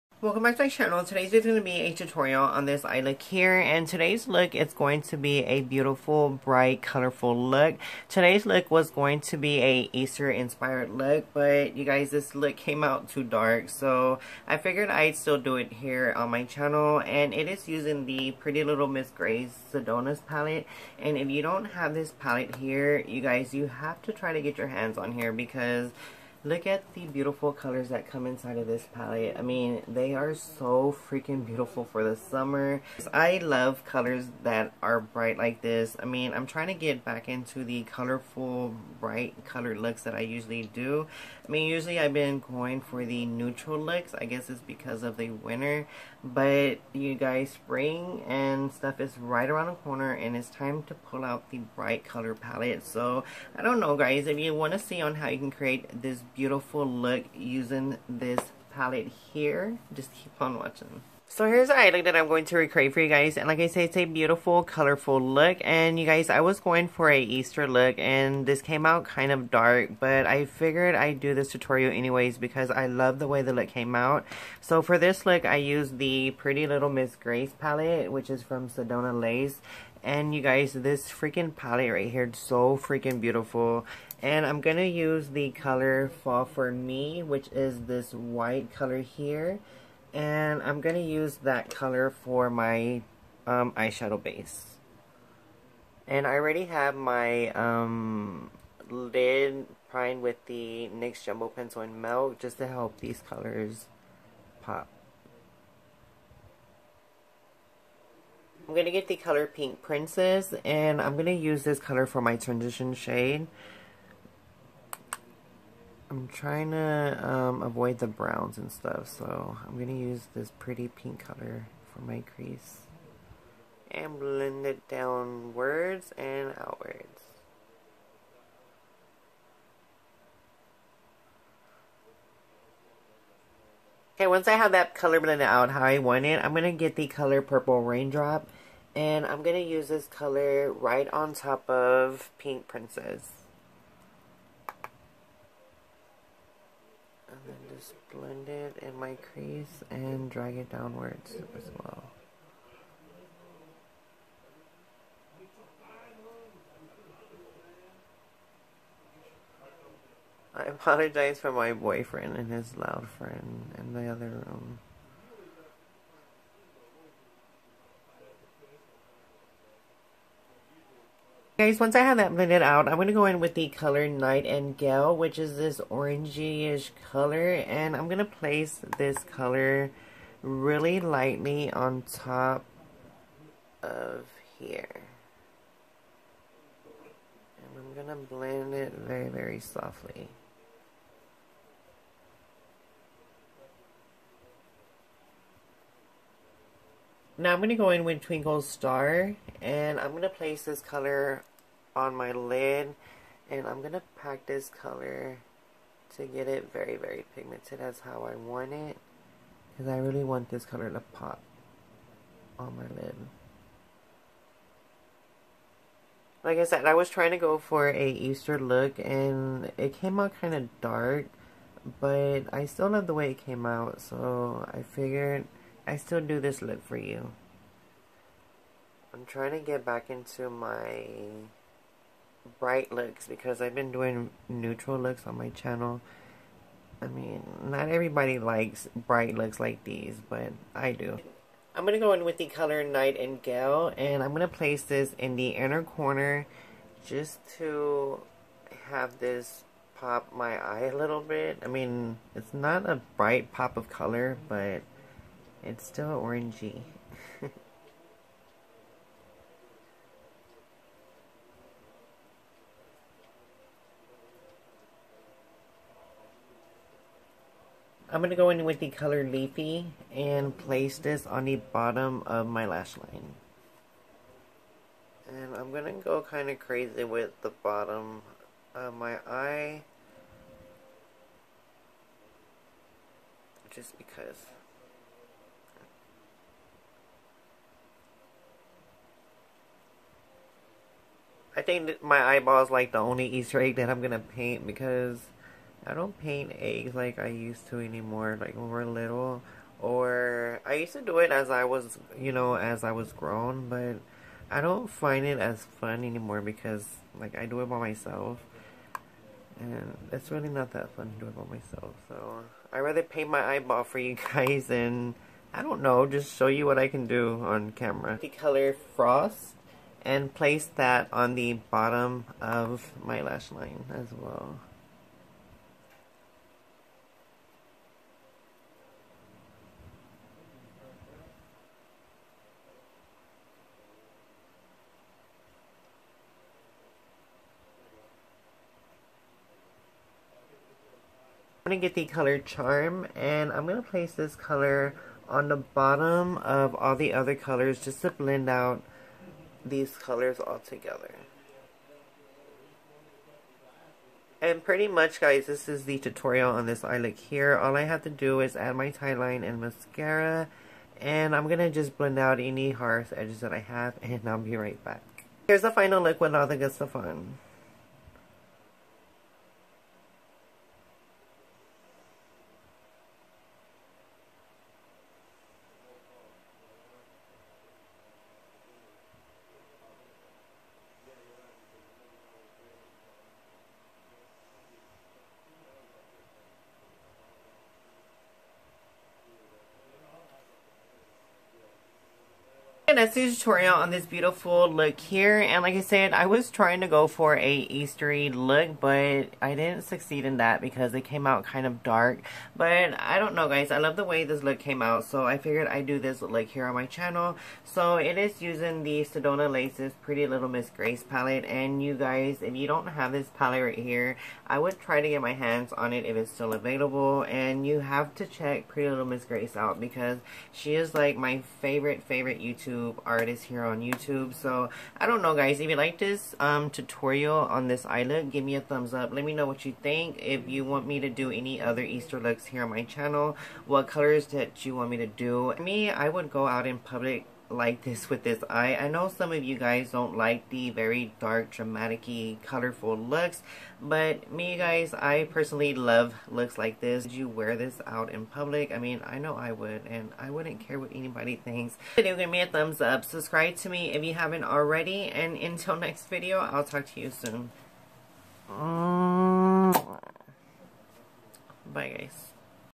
Colorful Spring eyes/Ft.Prettylilmzgrace plt
Hey loves here's a colorful look for the spring using the pretty lil mz grace Plt. OMG guys she's my favorite you tuber of all lol.. check her out Please remember to LIKE/COMMENT andSUBSCRIBE for more video's ♡